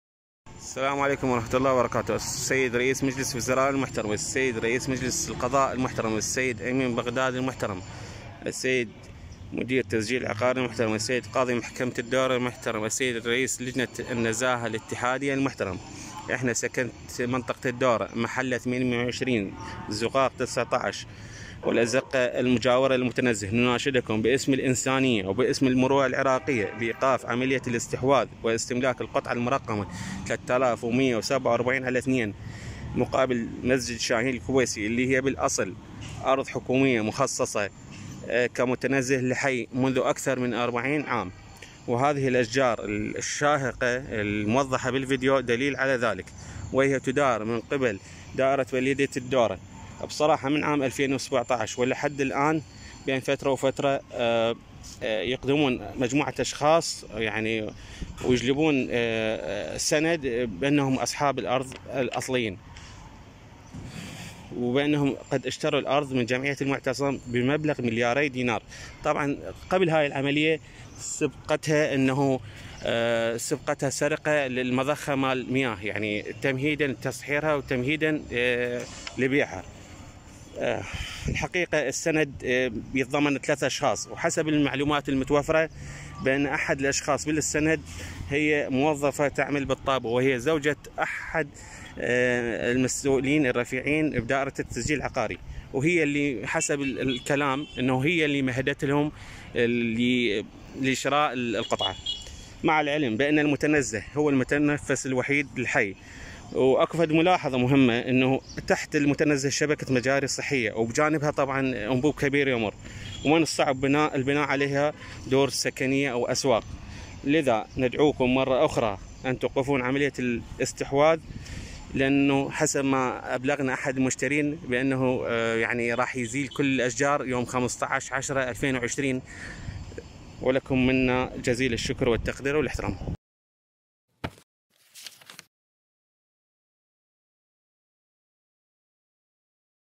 زوجة مسؤول بالتسجيل العقاري تستحوذ على ارض حكومية في بغداد
مناشدة عاجلة..  زوجة مسؤول بالتسجيل العقاري تستحوذ على أرض حكومية بالدورة في بغداد